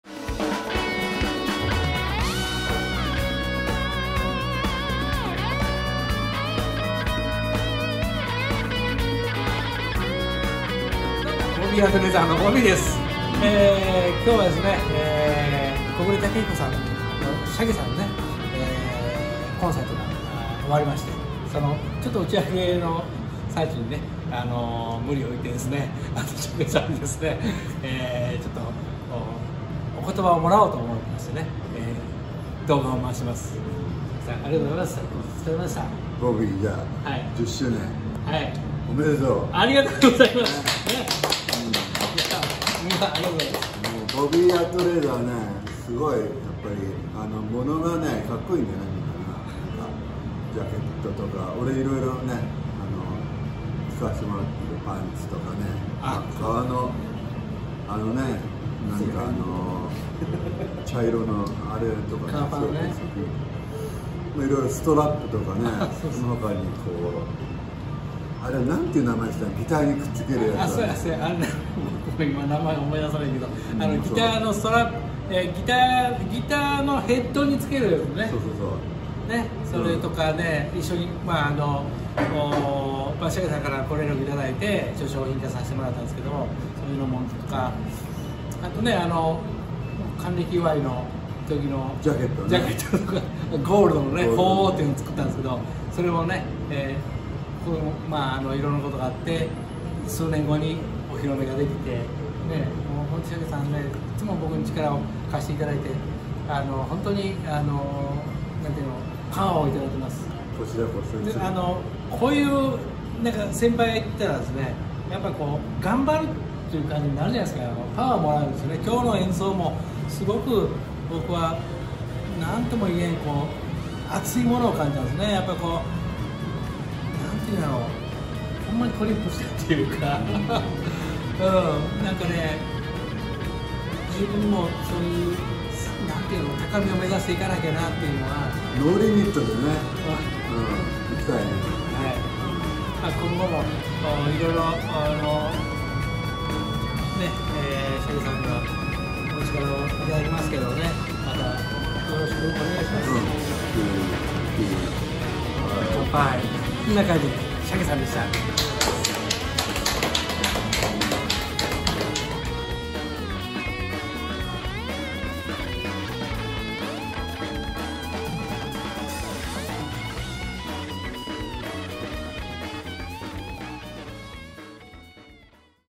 ギタリスト木暮”shake”武彦さんからボビーアートレザー®︎創業10周年スペシャル・メッセージ/Bobby Art Leather
ギタリスト木暮”shake”武彦さんからボビーアートレザー®︎創業10周年スペシャル・メッセージ